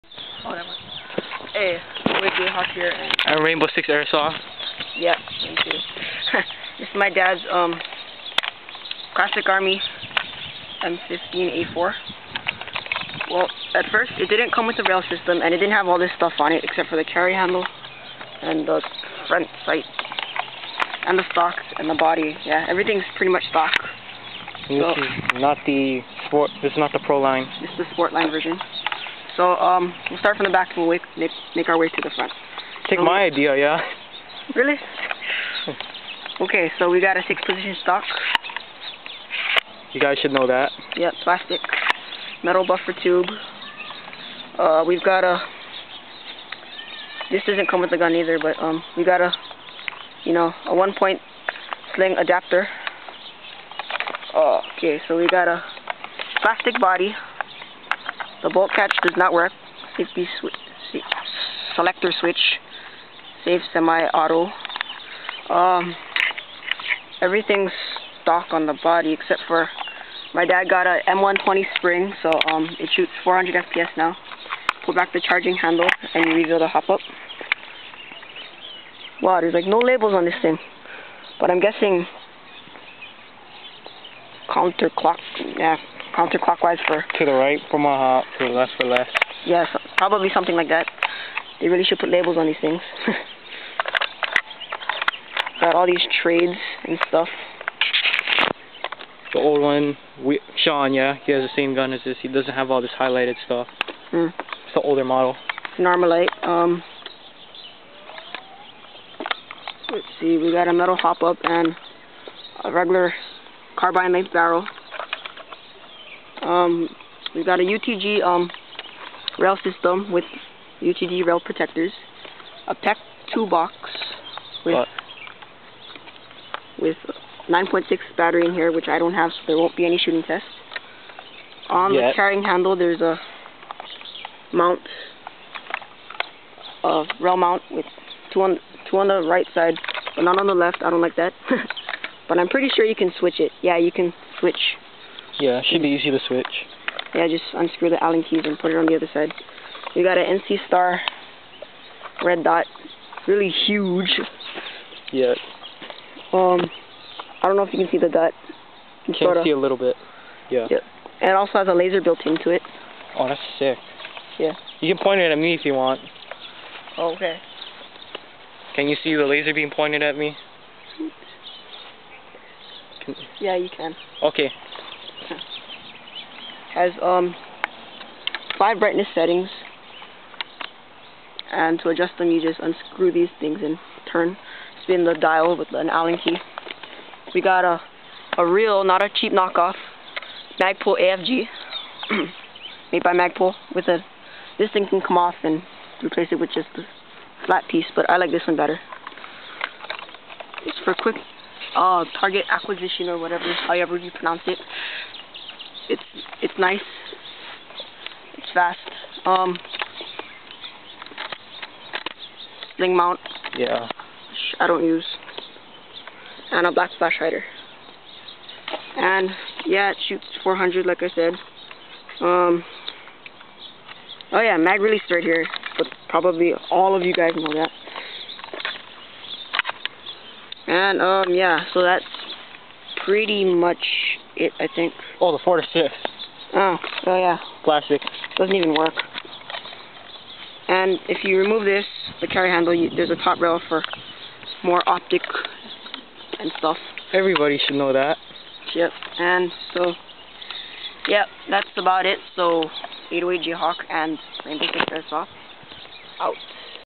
classic army m15a4 carbine (sport line version)
this gun is real awesome. it has a bunch of mods and accessories which include;
m120 spring
utg rail system w/ rail covers
magpul AFG
carry handle rail mount
nc star red dot/ laser scope